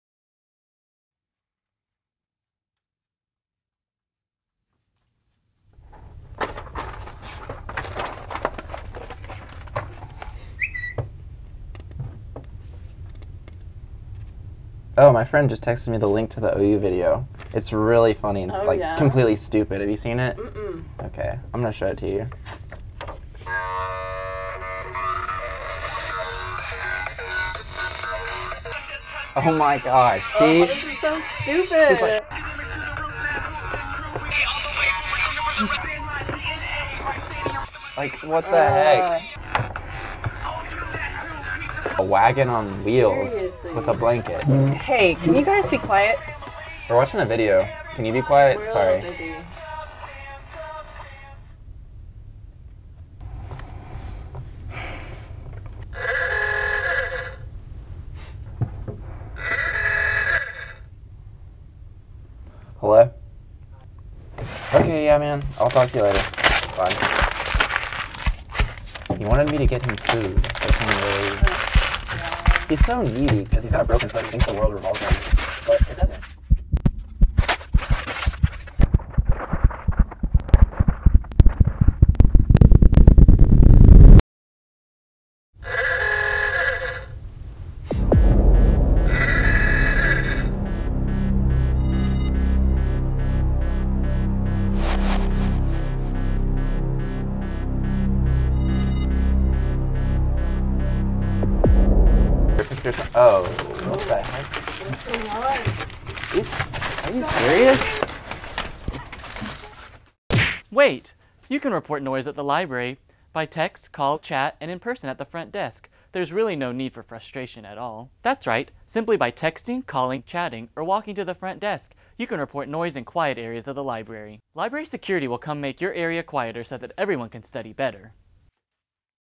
Don't Be That Guy...
Having trouble with that loud guy in quiet study? You can report noise at the library by text, call, chat or in person at the front desk.

Acting Credits:
Austin Bowles
Amber Parsoneault
Emily Nielsen

Voice Credit:
Garrett Hondronastas